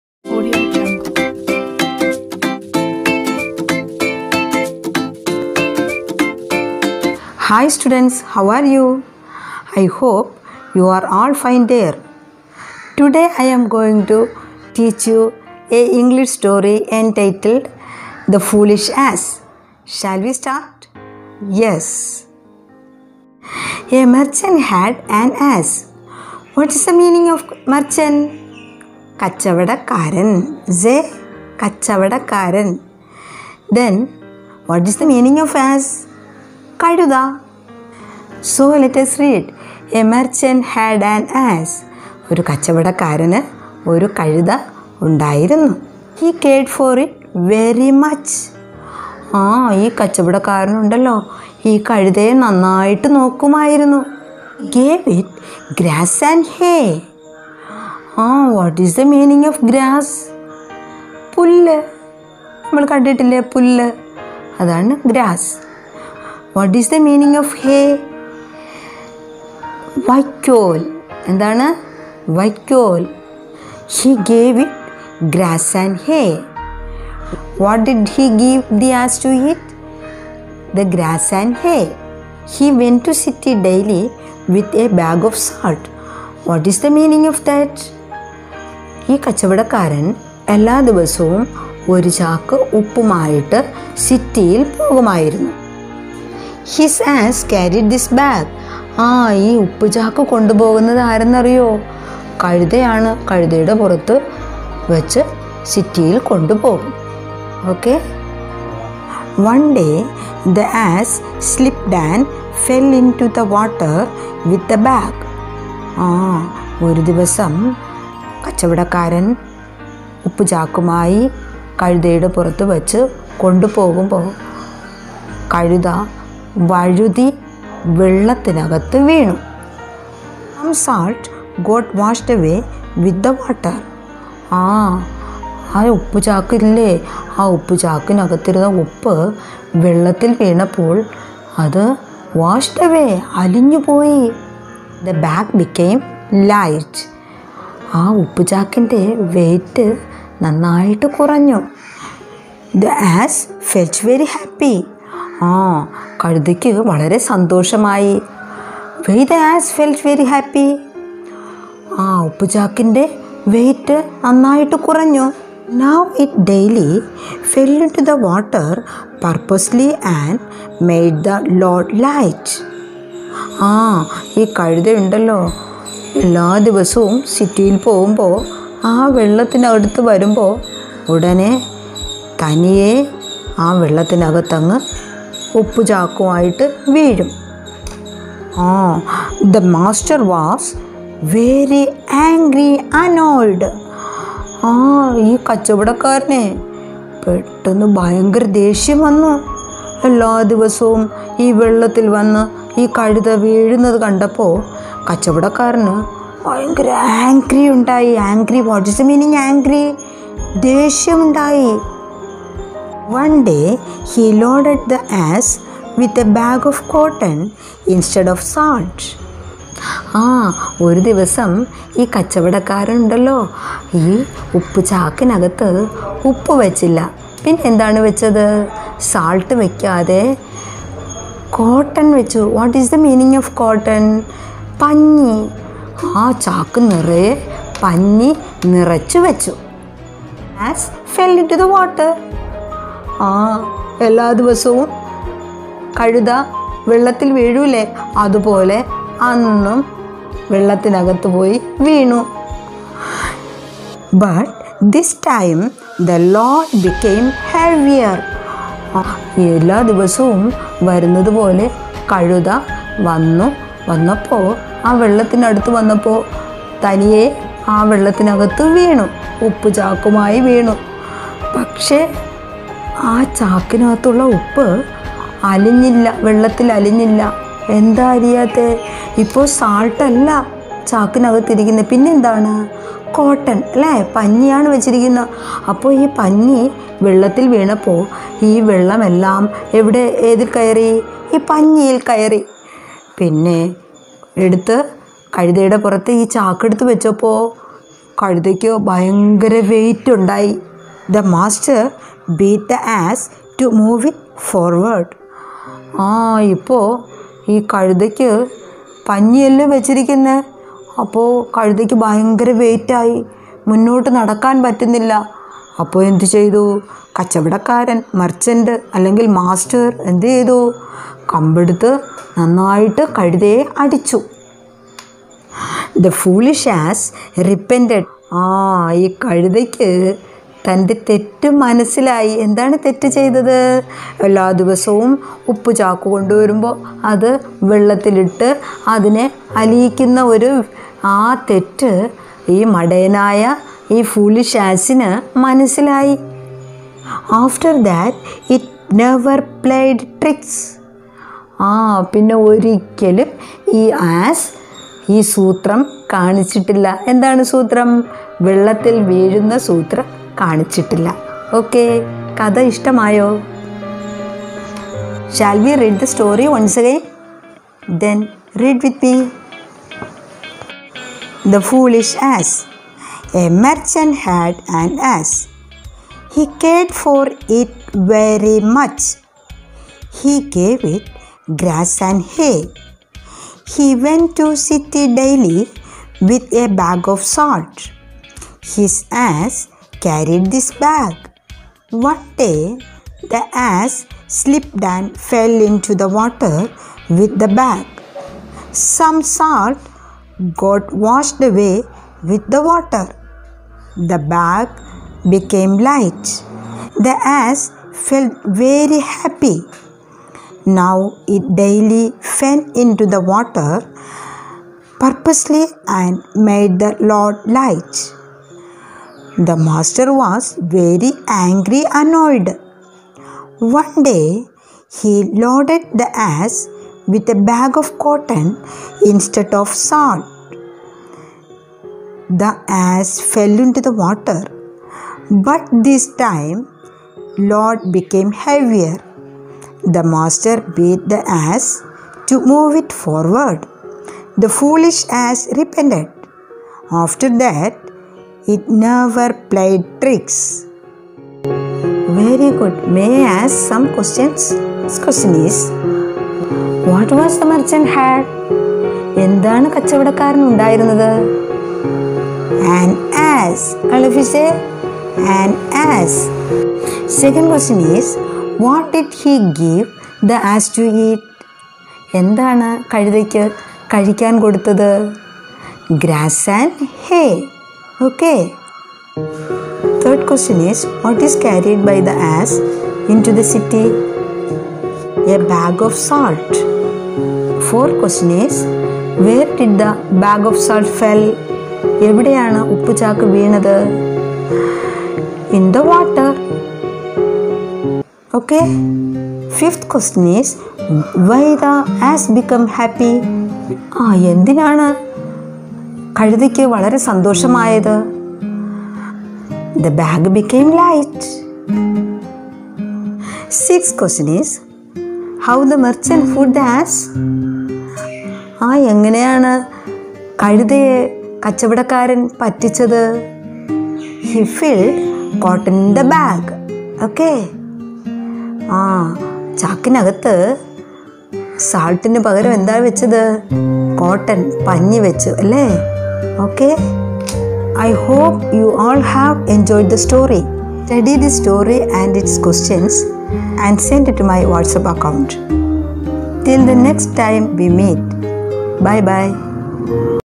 The Foolish Ass | Kids story | Narration and Reading
Dear Parents👨🏻‍🦰👩🏻‍🦰,
                 This video is made for our kids for easy learning of English stories with their meanings.Do watch and encourage our kids👶🏻👧🏻👦🏻 to study this story by-heart.